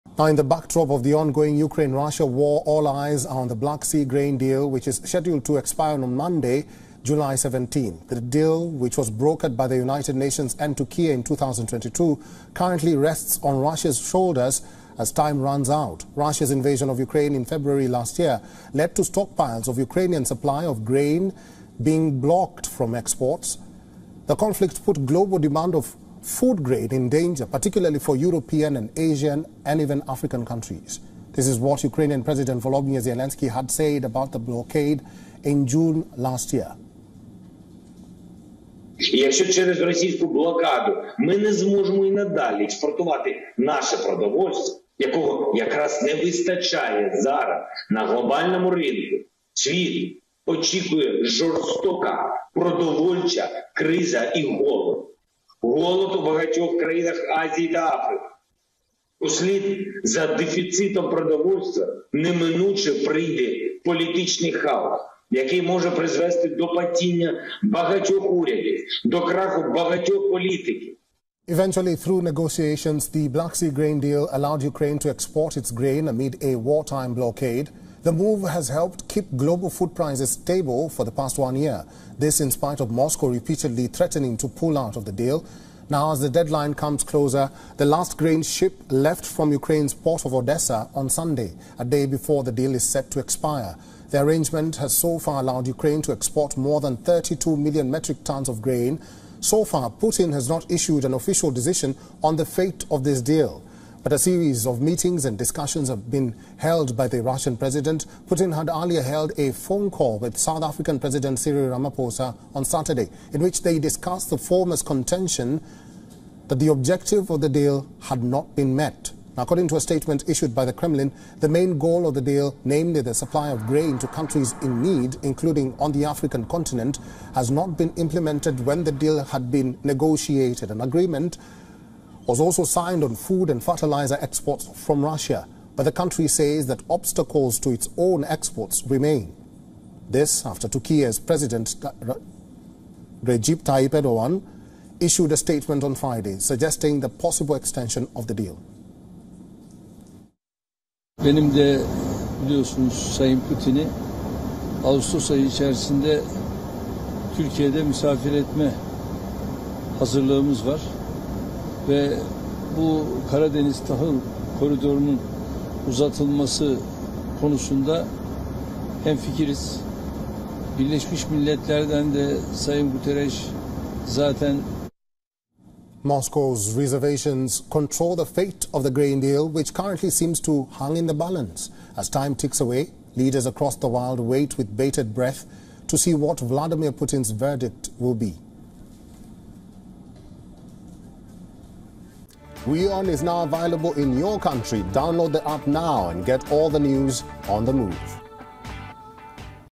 Kremlin: Main goal of Black Sea Grain deal has not been met | Latest World News | WION Pulse
The future of the black sea grain deal looks to be in the doldrums. While Turkiye's President Erdogan had on Friday confirmed the renewal of the deal. In a fresh development now, Russia’s Putin seems to have reiterated that he sees no ground to renew the deal. He apparently said this during a phone call with South African President Cyril Ramaphosa. The expiration date of the black sea grain deal has now been decided. Watch the video to know more.